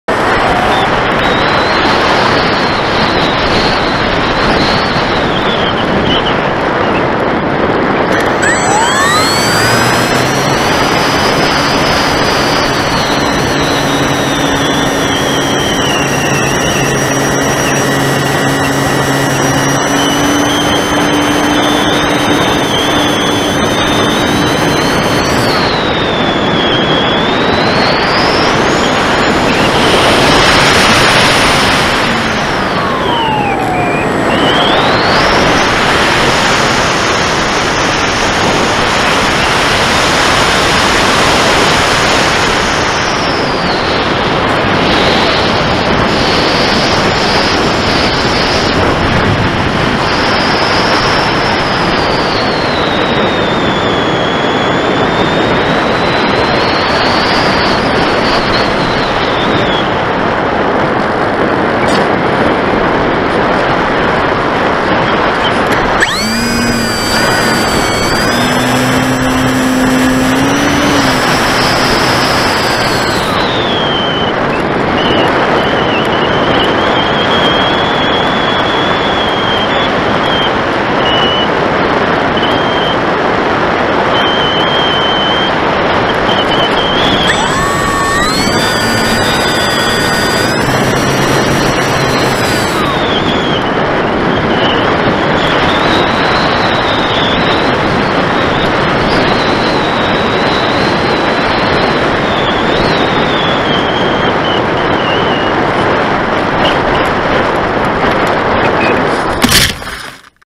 Top RC Lightning - Underwing Rear View Cam - Aerobatics & Landing !!
This is my TOP RC Lightning at the field on a beautiful afternoon !!  Just having a quick blast around the field, I really love flying this model...  It is so responsive, fast and aerobatic..  I just need to practice my aerobatics more ..    Haha...

I have upgraded the motor on this to the 1150kv running a 10x7 prop..  2200mah 3s battery, 1.5mt wingspan which makes the total flying weight 1050 gms.  Very sleek & low drag, it really glides on for ages and had heaps of climbing power...  I have tried a new underwing camera mount this time...  stand by for some more videos coming...  :-)